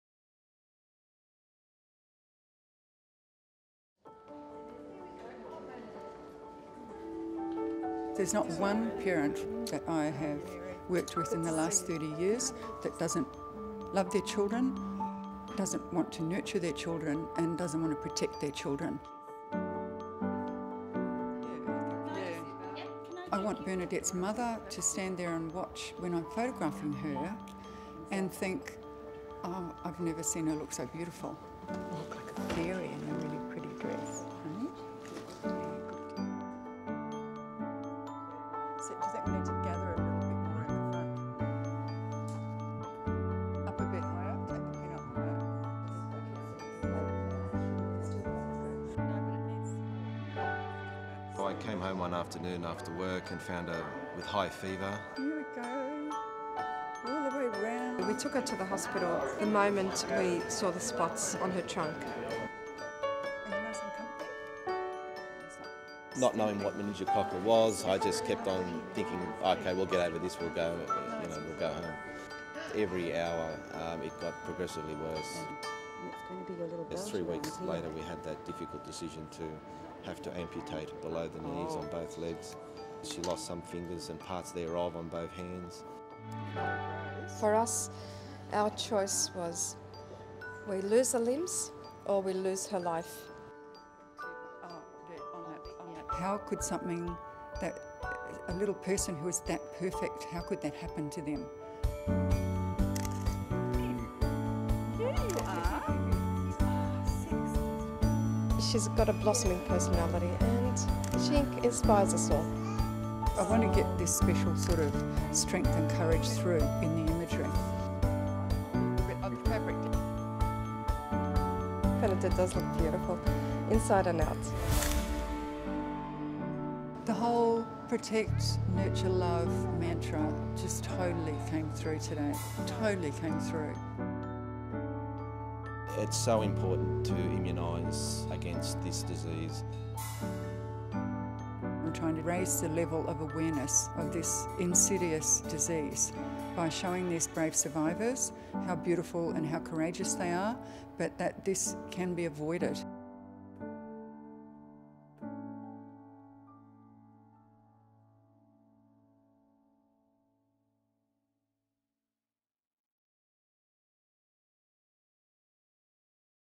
Protecting Our Tomorrows: Portraits of Meningococcal Disease (Sydney, Australia)
This video offers a behind the scenes look at the Protecting Our Tomorrows: Portraits of Meningococcal Disease project with Anne Geddes. Protecting Our Tomorrows aims to foster education around the impact of meningococcal disease, and encourage preventative measures to protect children against its devastating consequences. Over the next 12 months, Anne will travel three continents to photograph survivors and families that have been impacted by the disease. Their images and stories will be shared in an eBook on World Meningitis Day 2014. The project is supported by Novartis Vaccines and Diagnostics.